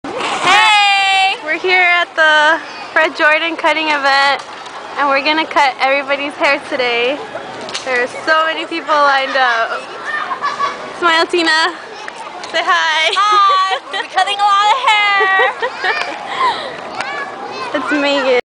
Paul Mitchell w/ fred jordan mission
Cutting hair for  children that are  on budget.
 Paul mitchell sent   people to cut hair for free .
Foot locker was also there.
 this was my 2nd time doing an event for fred jordan .